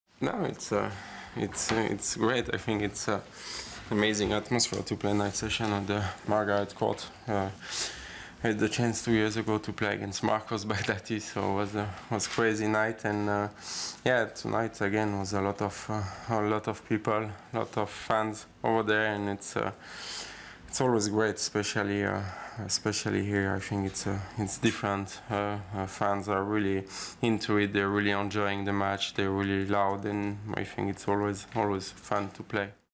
Margaret Court Arena atmosphere is special, says Wawrinka
World number eight Stanislas Wawrinka praised the crowd in the Margaret Court Arena who watched the Swiss beat Alejandro Falla of Colombia 6-3, 6-3, 6-7, 6-4 in the Australian Open second round on Wednesday. Wawrinka will face Canadian Vasek Pospisil in the next stage.